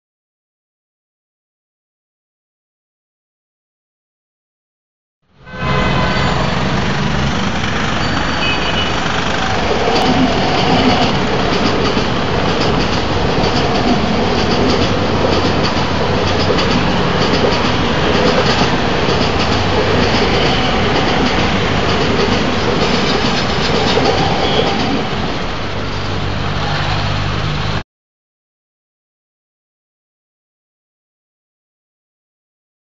Local Delhi - Sonipat Bound EMU
This is a video of local delhi - sonipat bound EMU passing up as well as speeding up under the Bridge near Karnal Bypass in Delhi.